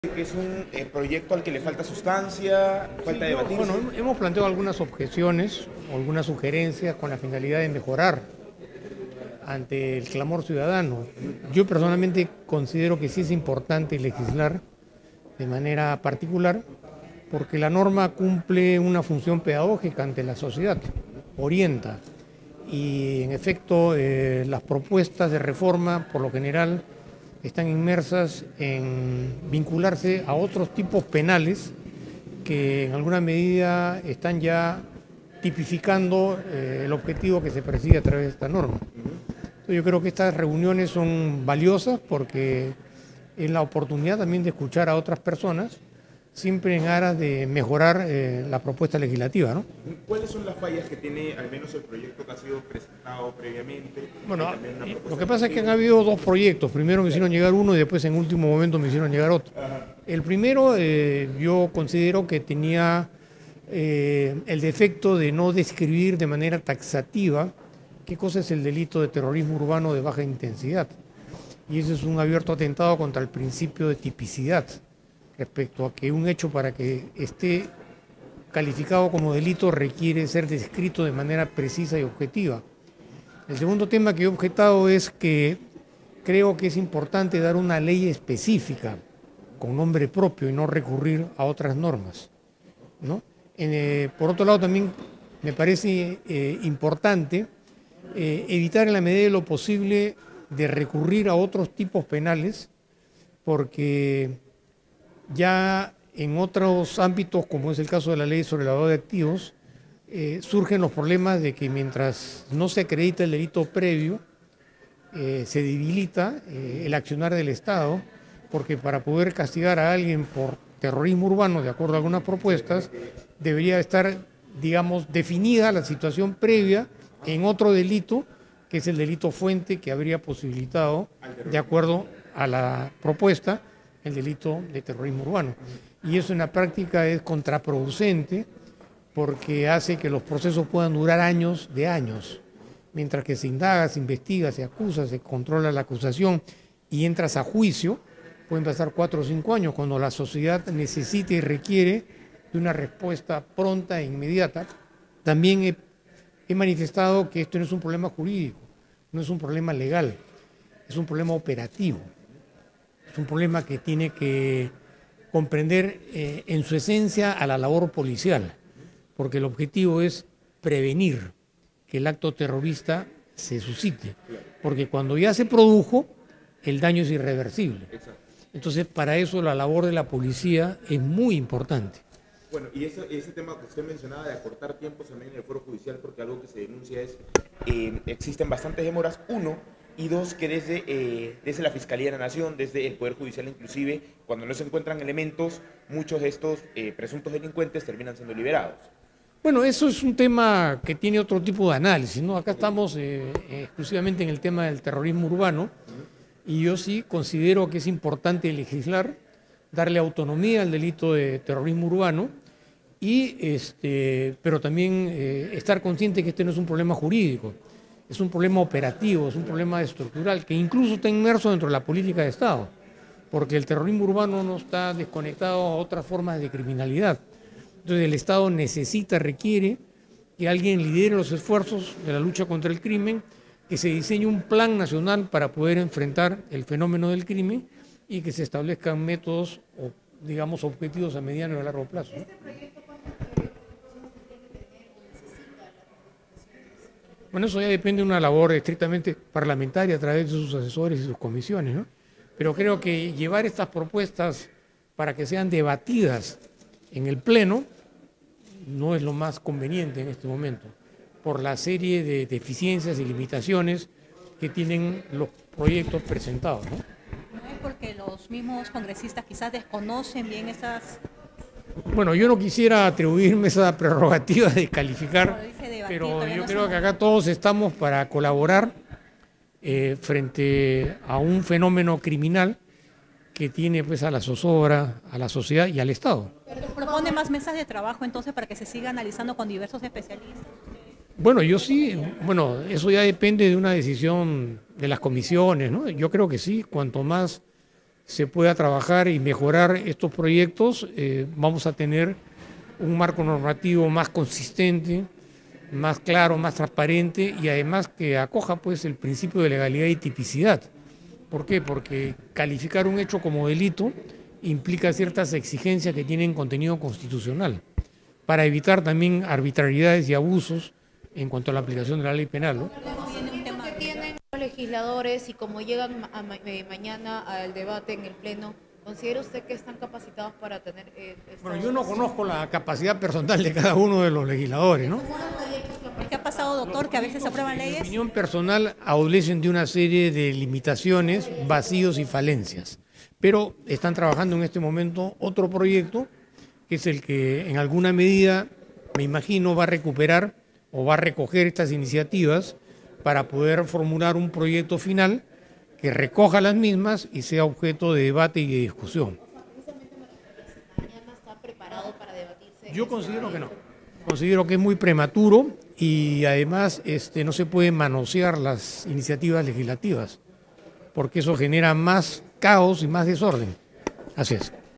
Luis Lamas Puccio sobre dictamen que tipifica el delito de terrorismo urbano
Expreso, últimas noticias del Perú y el mundo